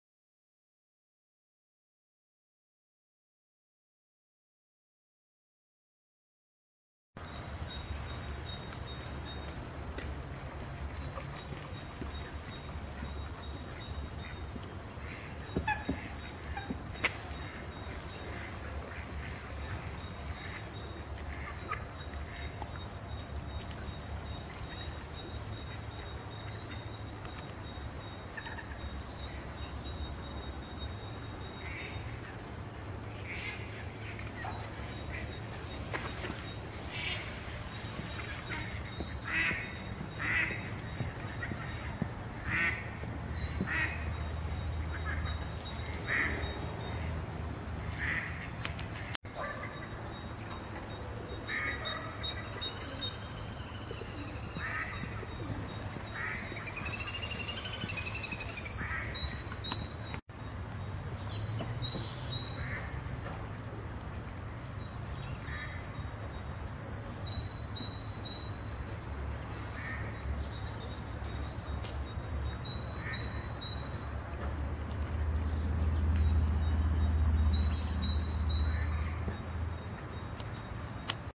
cigno
Cigno dall'aria altera (e anche un po' antipatica)